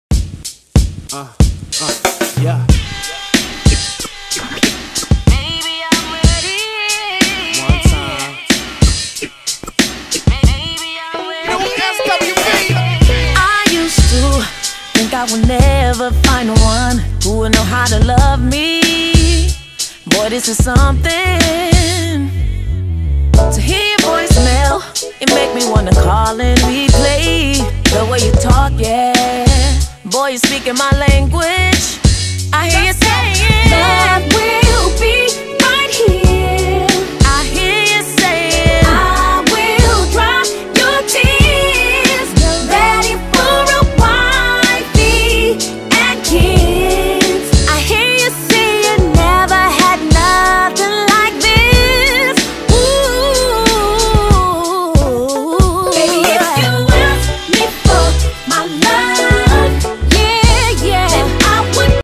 IM3Media produced Lelee Lyons of SWV book release party
Lelee Lyons of R&B group SWV released her new book I Regret The Day... Sept 4,2019